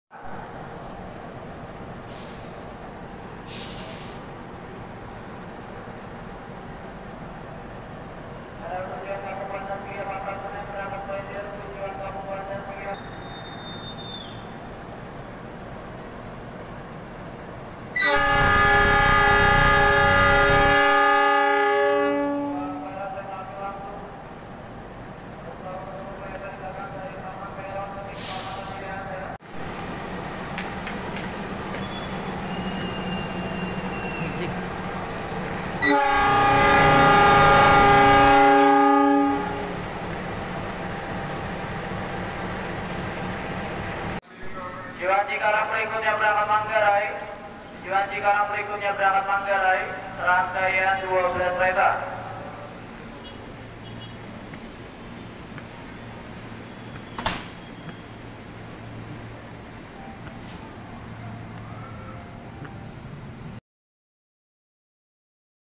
klakson CC 206 pas mau berangkat dan masinis tutup pintu 👍🗿
hal seru di jng